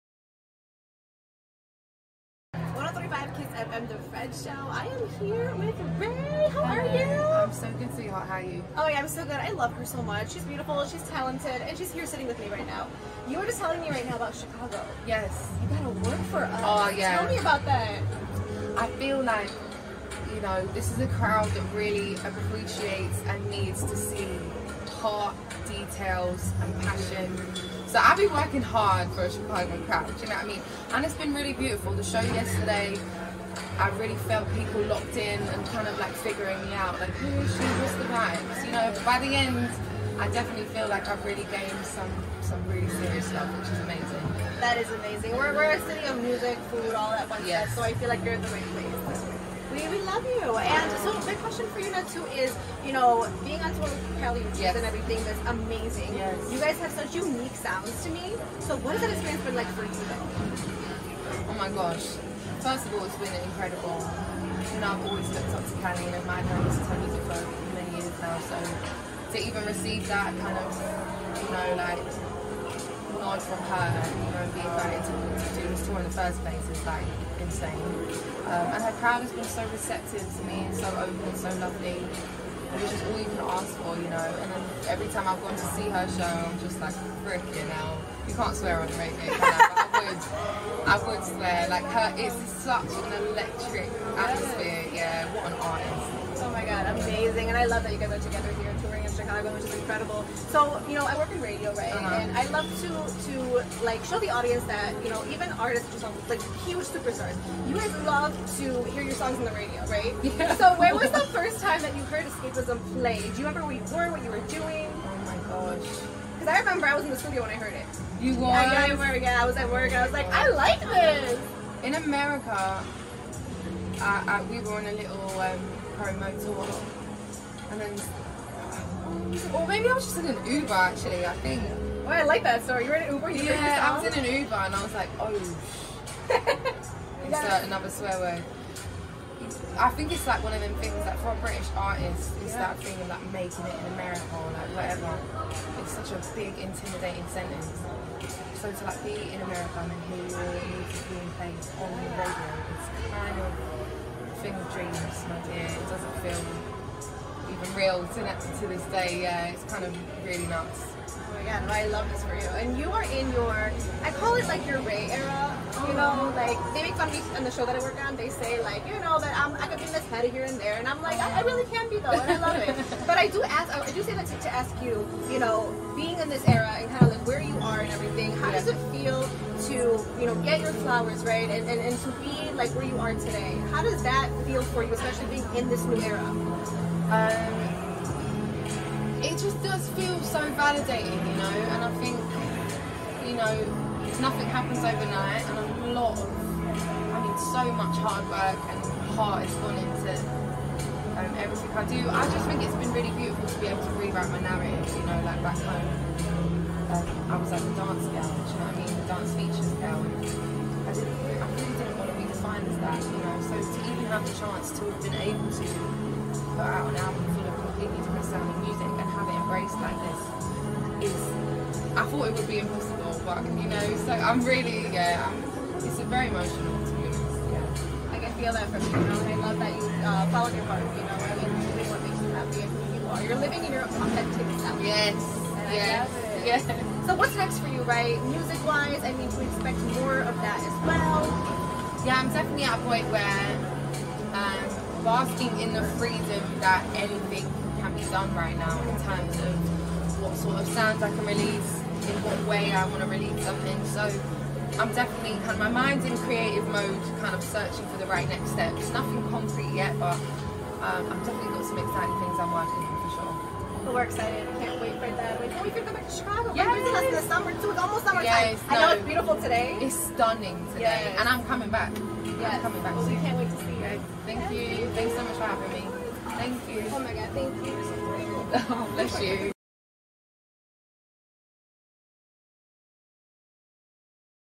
Paulina Interviews Raye
Paulina of The Fred Show interviews Raye about performing in Chicago, hearing her song on the radio, and more!